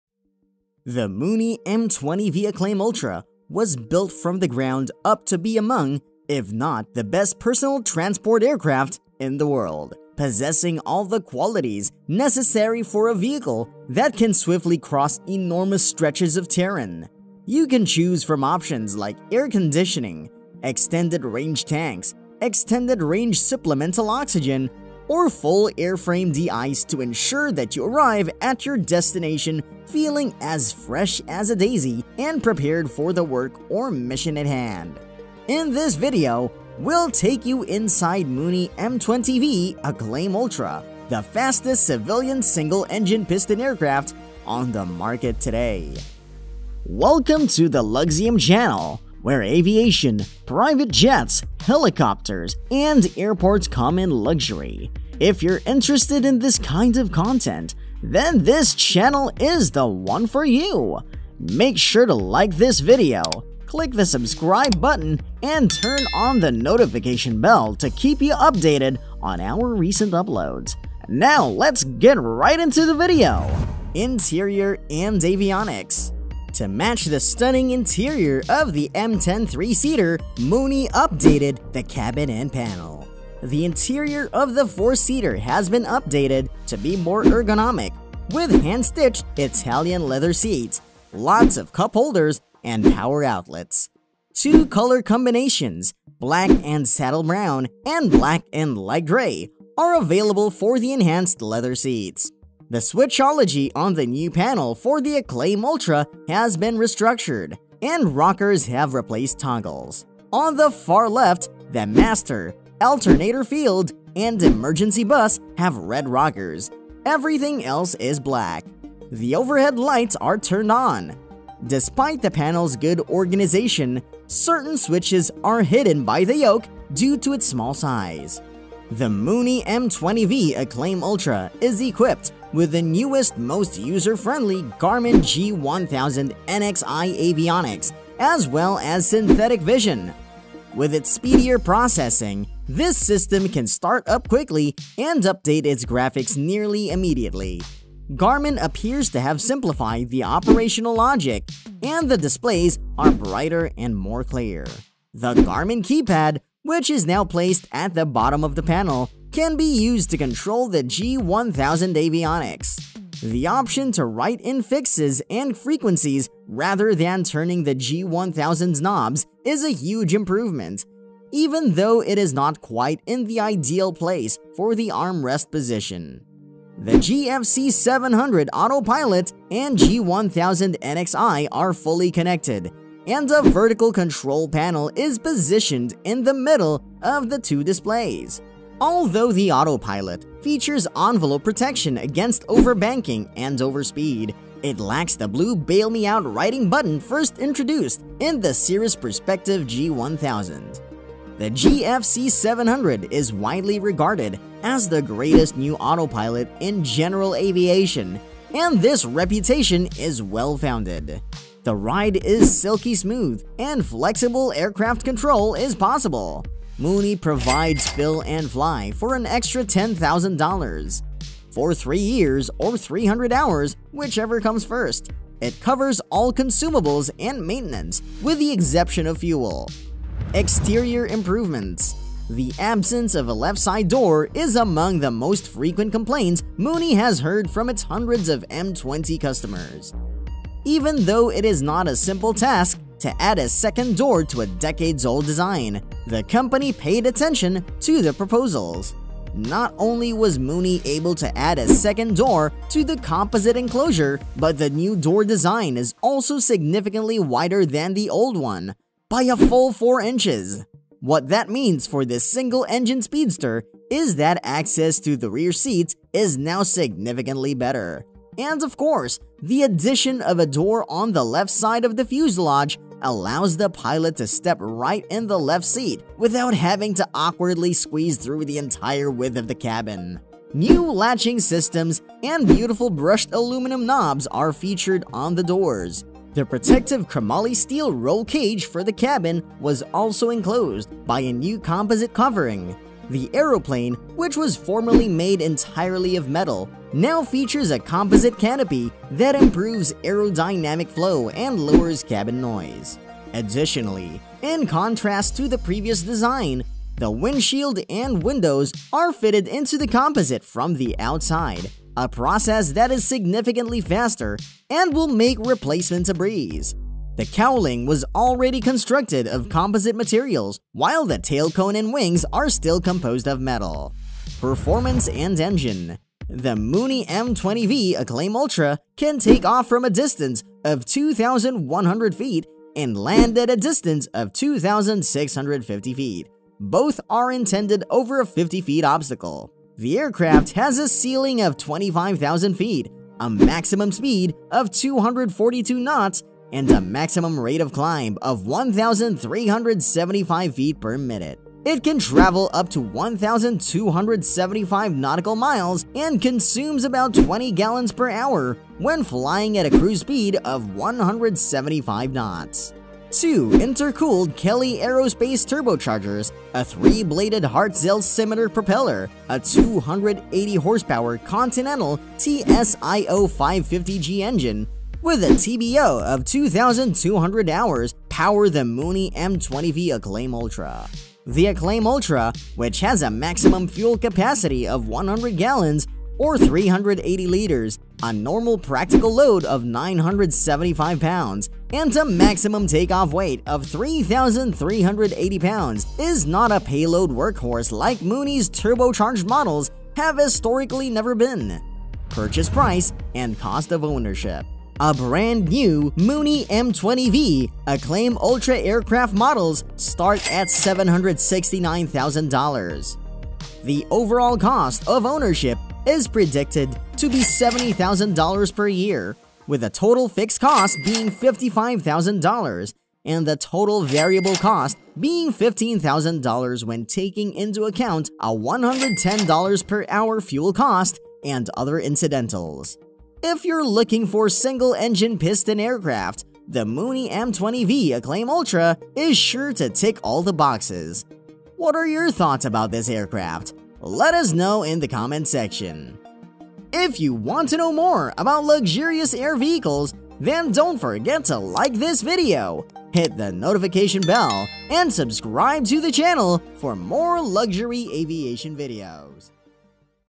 Inside $1 Million Mooney M20V Acclaim Ultra | Fastest Single-Engine Piston
The industry-leading M20 Acclaim piston single has undergone a redesign and update by Mooney International. Since being acquired by China's Meijing Group in 2013, this high-performance aircraft's launch represents the company's first significant upgrade. A second door , a composite-wrapped cabin, a clean-sheet interior, and a Garmin G1000 glass cockpit are the  improvements included in the updated model. In this video we'll take you inside of the Mooney M20V Acclaim Ultra, Fastest Single-Engine Piston Aircraft on the market today!